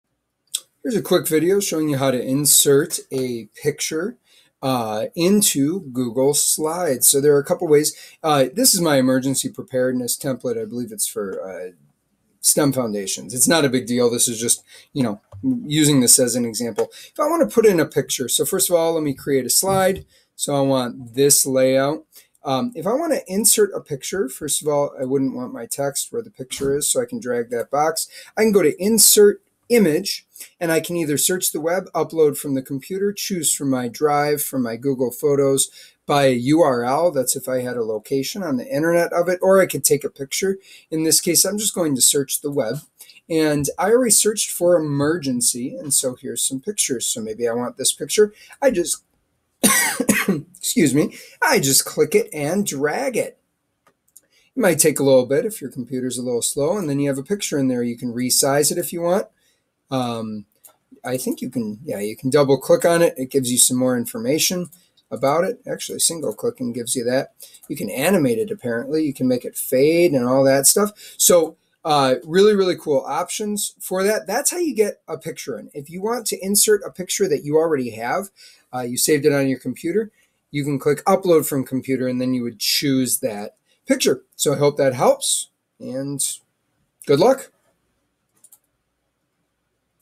How to Insert a Picture in Google Slides
Here's a quick video showing you how to insert a picture in Google Slides.